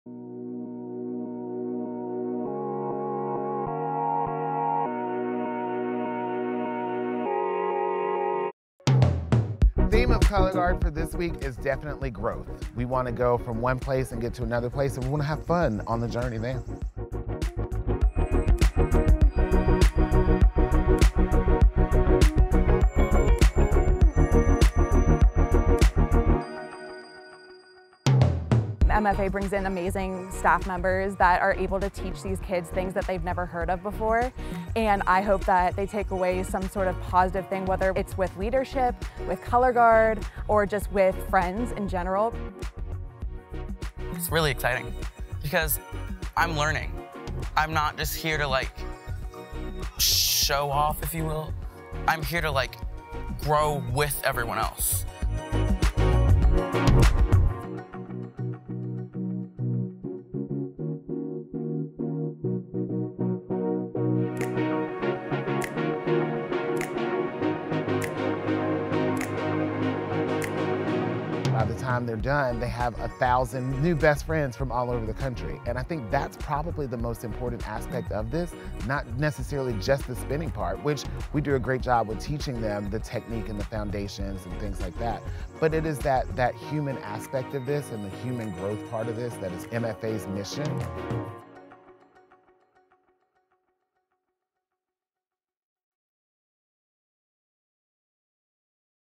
The Color Guard Division at Summer Symposium, presented by Yamaha
Learn from leading instructors and immerse yourself in hands-on experiential learning opportunities. The Symposium brings together motivated students, rockstars of color guard education, and training in leadership. From beginners to advanced students, the Color Guard Division meets students where they are.